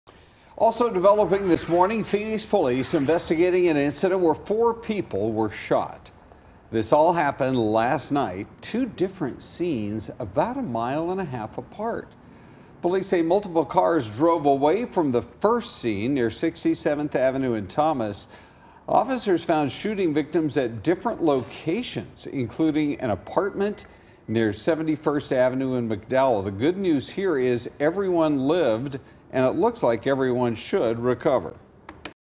4 Phoenix shooting victims found nearly 2 miles from crime scene
Officers first responded to the shooting and learned several cars drove away from the scene. Then, four victims were found in different parts of the area, including at an apartment complex near 71st Avenue and McDowell Road – about a mile and a half away.